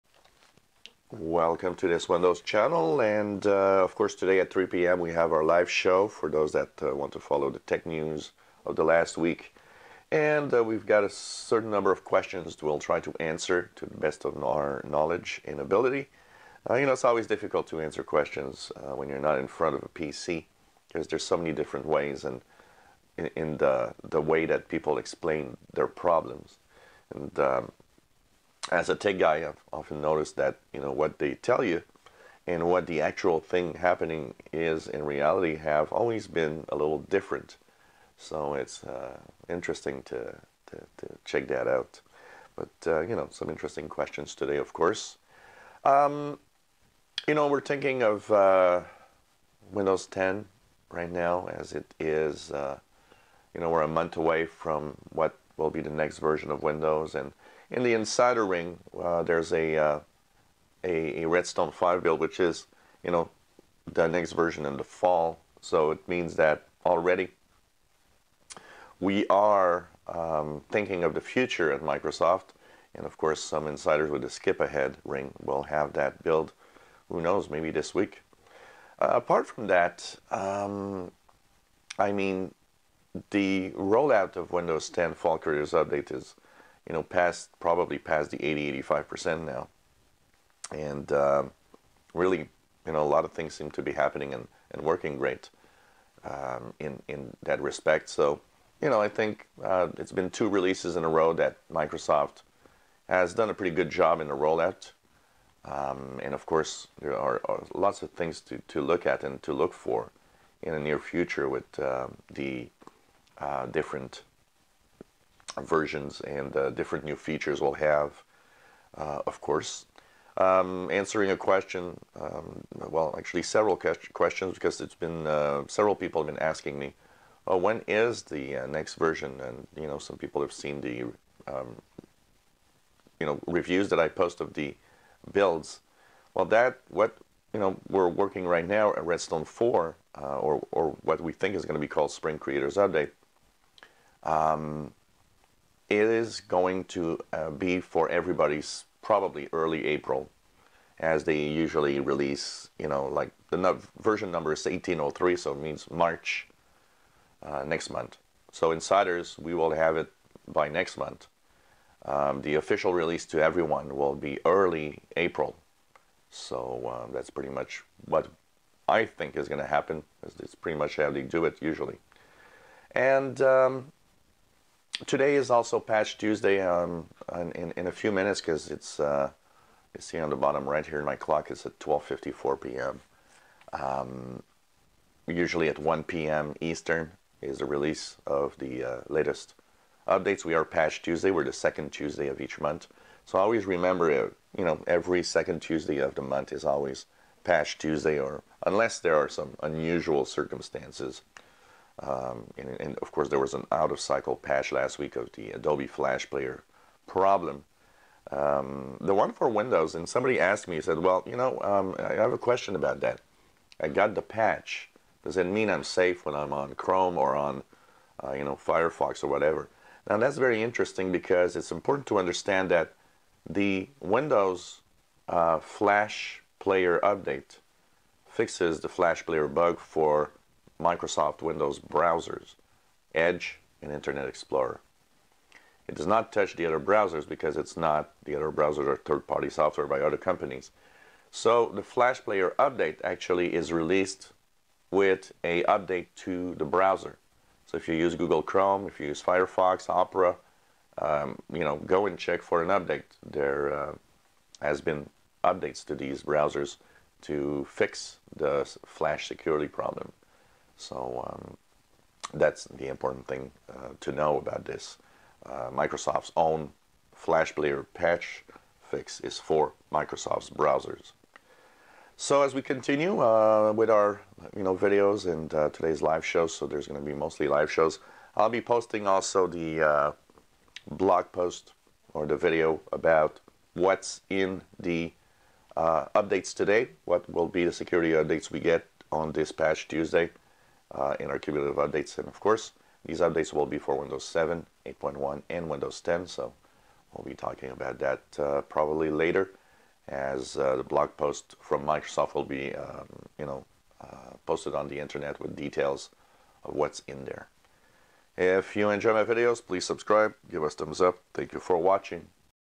Windows 10 personal observations patch tuesday and next version of Windows
Some personal observations, answering questions